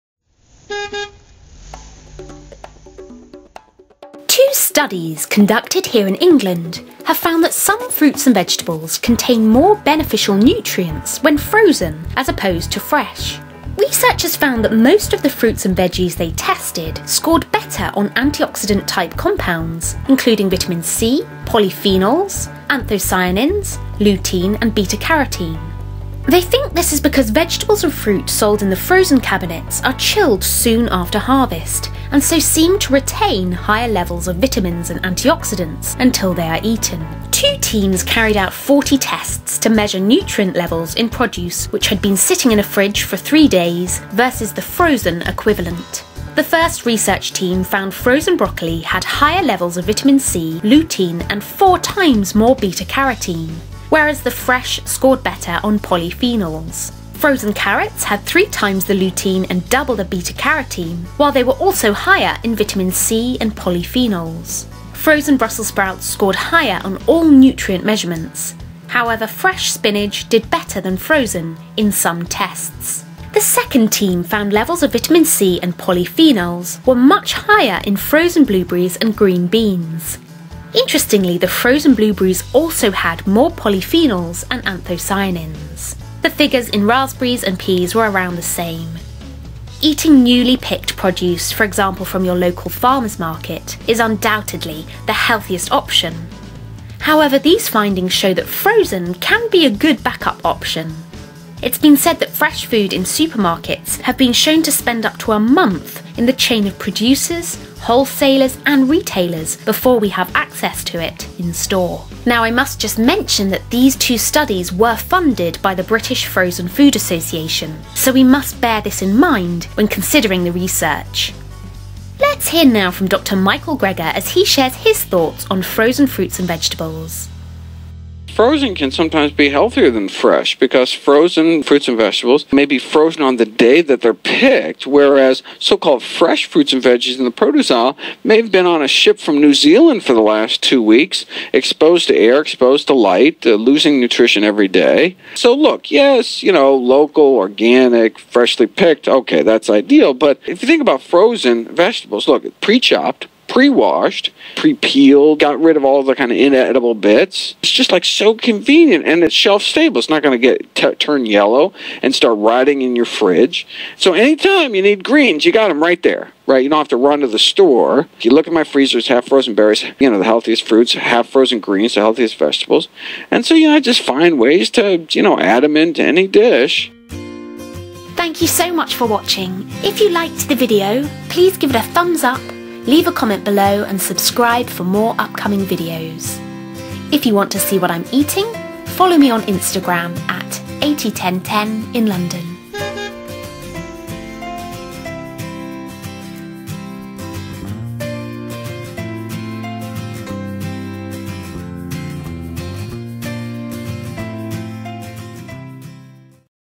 Should We Avoid Frozen Fruits & Vegetables? Dr Michael Greger
Should we avoid eating frozen fruits and vegetables? Has frozen fruit and veg lost it's nutrition? Do nutrients get destroyed when we freeze food? Should we only eat fresh fruit and veg? Is there any produce we can freeze? Is there any produce we should never freeze? Keep listening as Dr Michael Greger and I answer these questions.

DR GREGER'S NEW BOOK IS OUT!!!